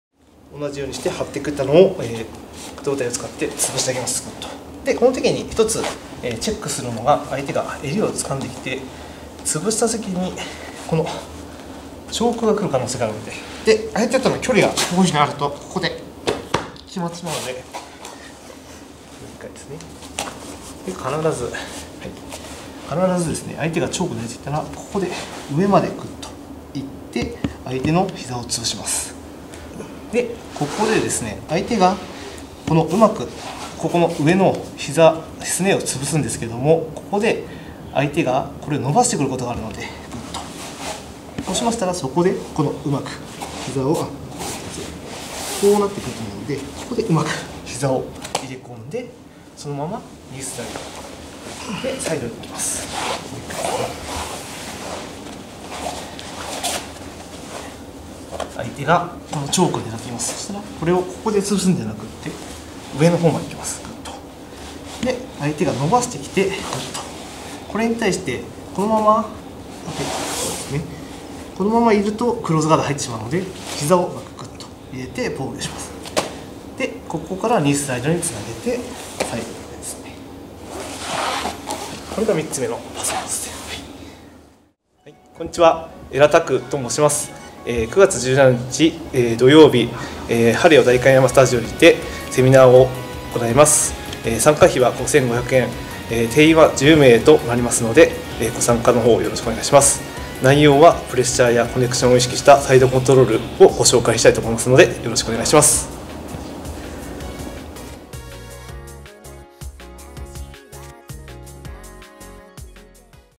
【ブラジリアン柔術】ニーシールドに対するパスガード⑥ / 江良拓【ミューズ柔術アカデミー】
江良拓セミナー「プレッシャー＆コネクション」
■日程
9/17 (土) 14時 - 16時
■場所
HALEO代官山スタジオ
(渋谷区猿楽町24-1 ルーブ２)
■参加費用
5500円(定員10名)